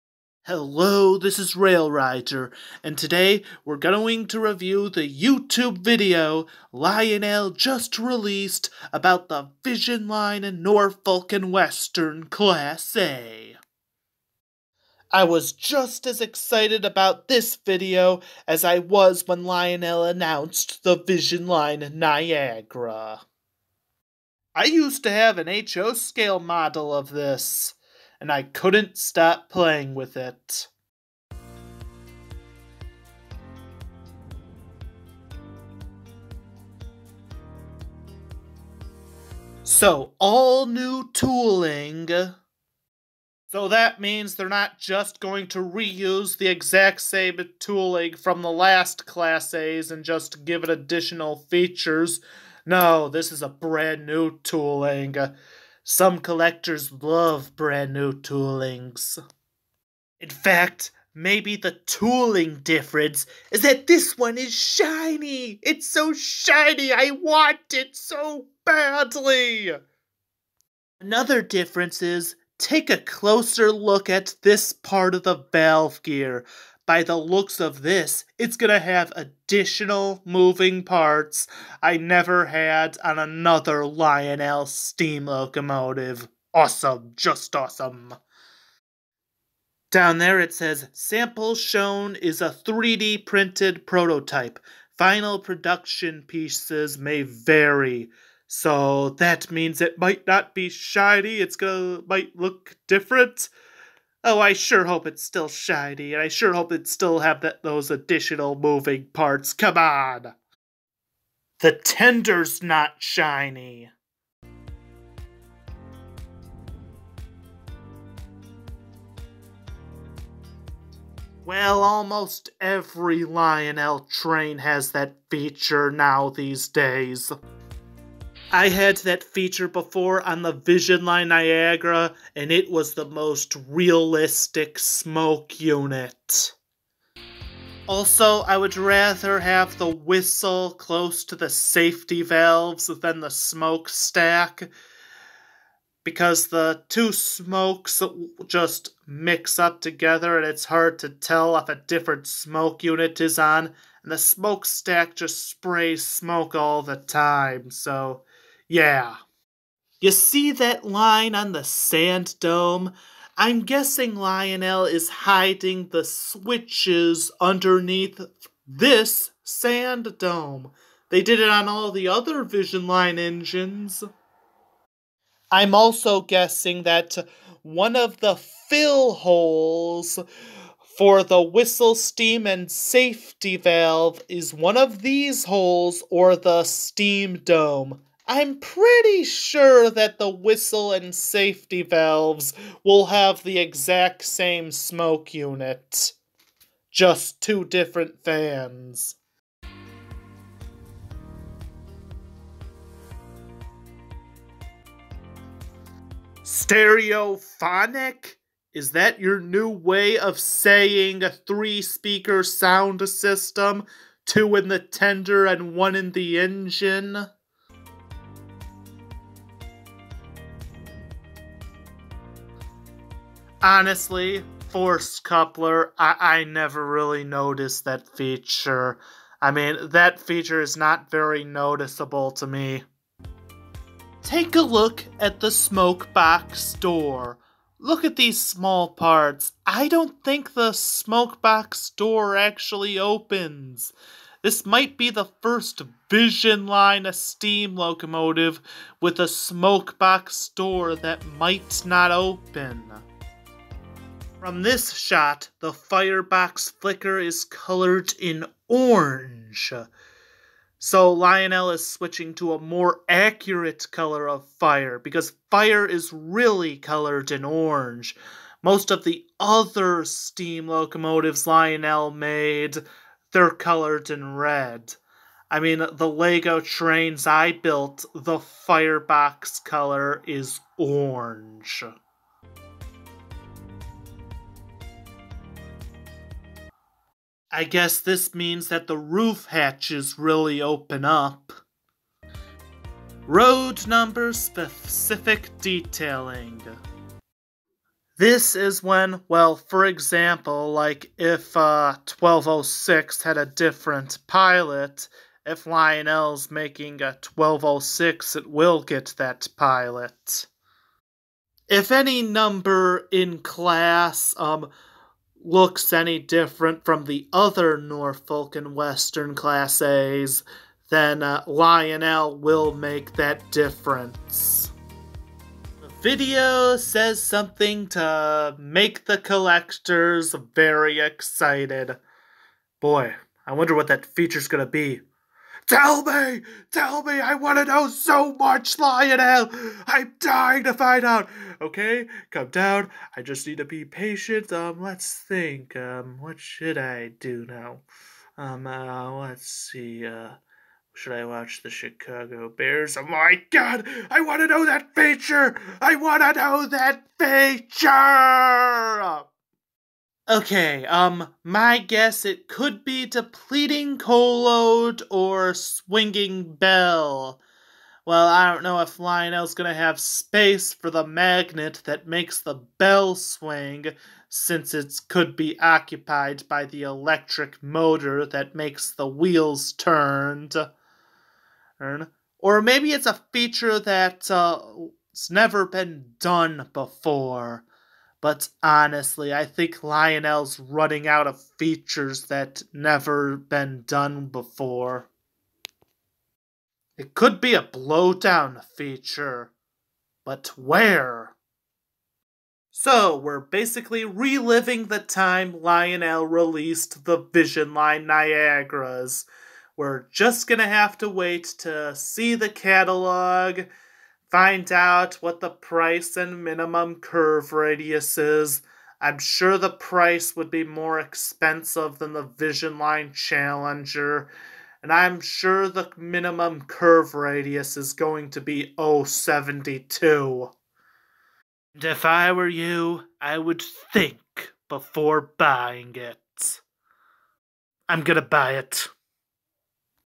Lionel Vision Line Class A Video Review
Lets talk about this exciting new announcement by Lionel.